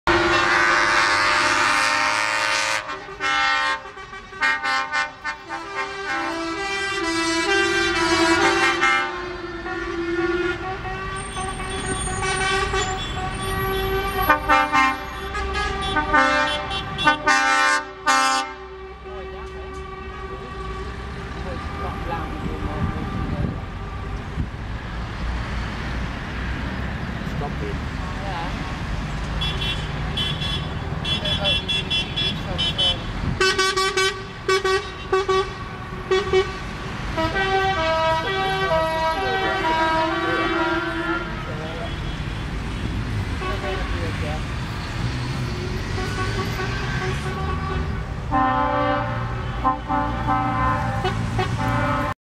Vrachtwagens rit
6 Juni de vrachtwagens rijden weer een route met verstandelijke gehandicapten
Een super iets voor deze mensen en hun fam.